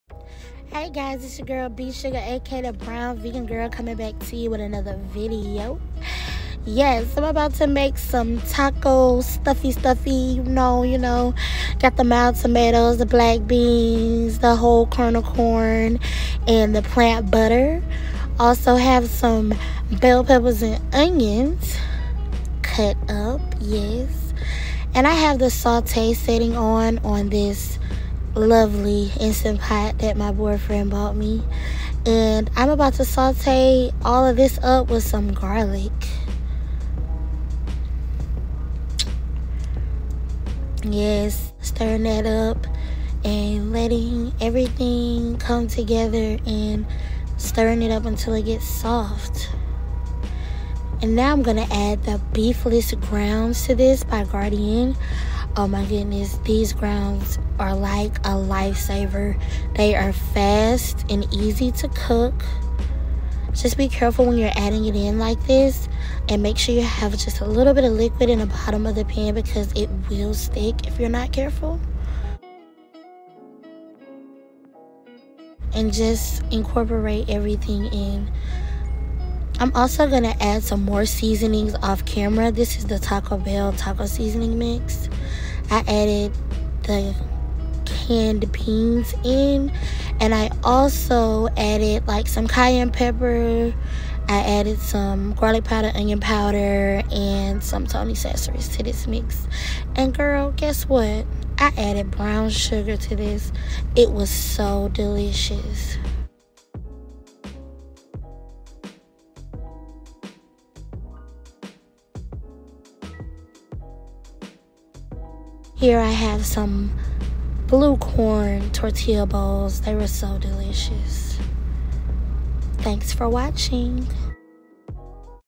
Vegan Taco Salad Made In My Big Rig!
This instant pot came from Walmart. It’s the Pioneer woman’s one.